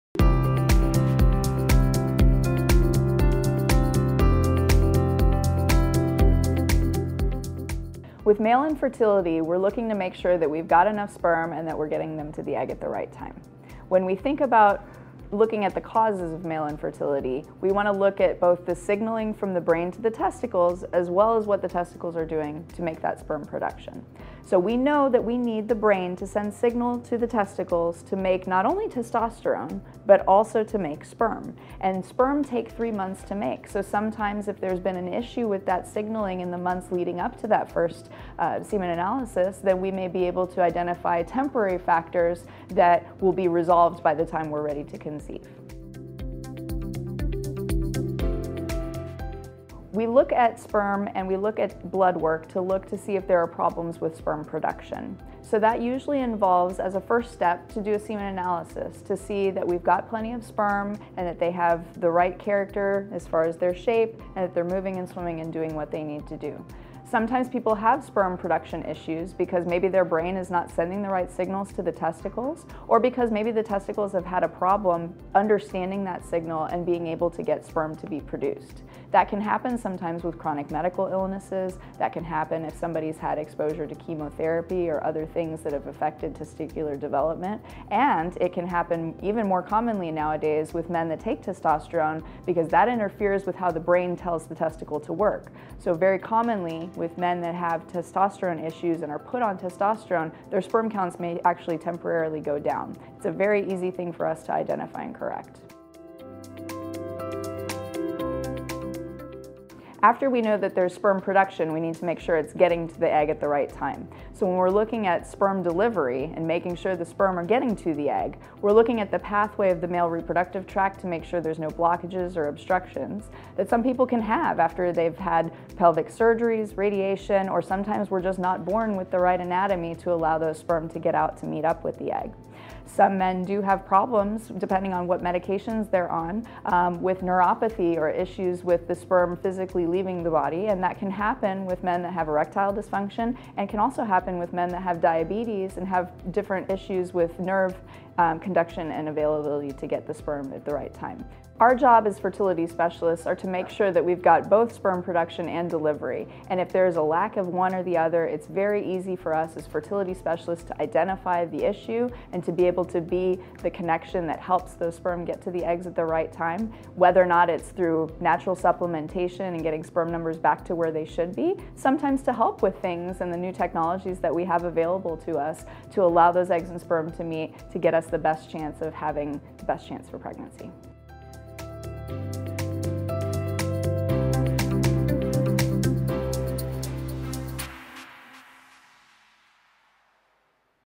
Dr. Erika Munch on Understanding Male Infertility & How Hormones and Sperm Function
Male infertility affects about half of the couples seeking treatment, with many men showing no symptoms. That's why Dr. Erika Munch, fertility specialists at Texas Fertility Center San Antonio, assesses the male partner of every couple who visits our practice.

In this video dr. Munch explains about male infertility, males hormones and sperm function.